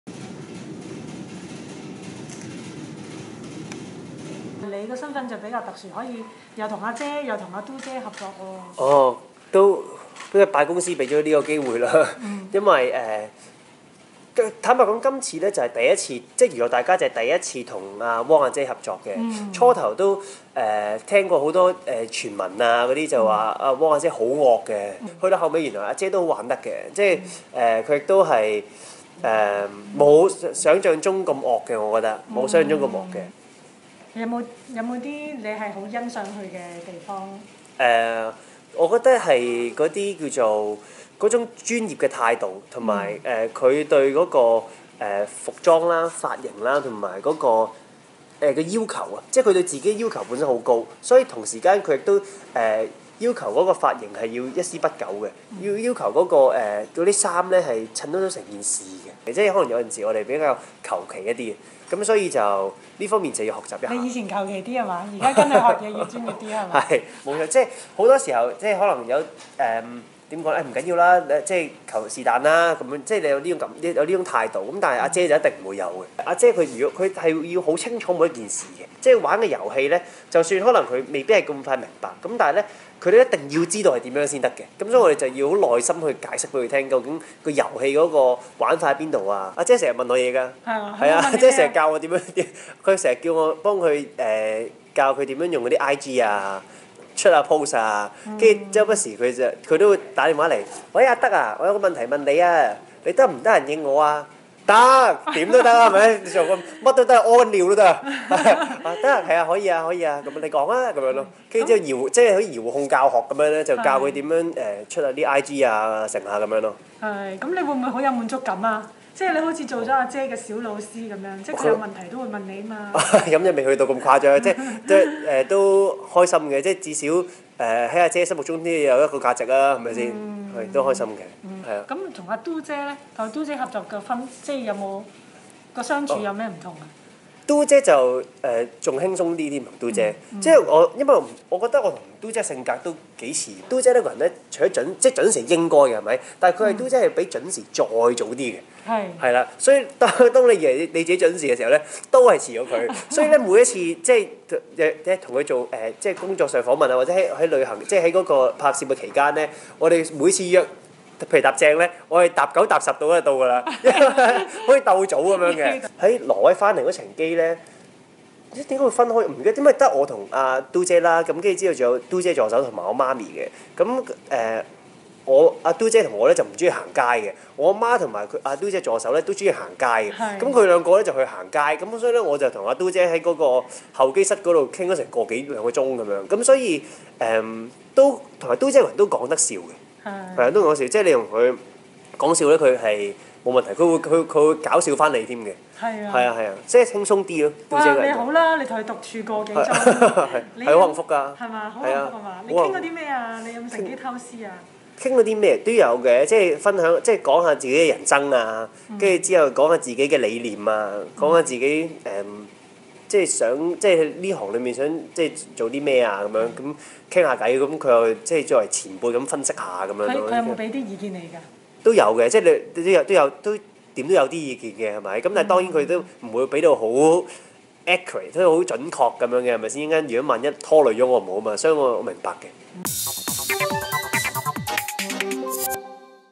【專訪】教汪明荃玩社交網 余德丞︰ 我在她心裏有點價值
余德丞（Dickson）前年踢波期間昏迷險送命，大步檻過後早已回復狀態，在演藝事業上再度出發，有份主持的《娛樂大家》系列、《台北101種玩法》及《X偏方 全民拆解II》連環推出。

回想過去一年，Dickson先後跟兩大阿姐汪明荃（阿姐）、鄭裕玲（Do姐）合作，獲益不少。「傳聞阿姐好惡，其實她會開玩笑，做事時認真、專業，值得敬佩﹗她注重服裝、髮型，即使是綵排遊戲都會問清楚。我被她感染，做事不再求其。阿姐很好學，會向我了解手機程式、社交網發文的步驟。我在她心目中有點價值，很開心。」

至於Do姐，Dickson難忘拍攝《晚間看地球》時，兩人在挪威機場候機室單獨相處個幾鐘。「我們傾工作、講笑話，她又給我意見。我似Do姐一樣心急，她很準時，我會跟她鬥早。我們相處時幾輕鬆，Do姐愛開玩笑。」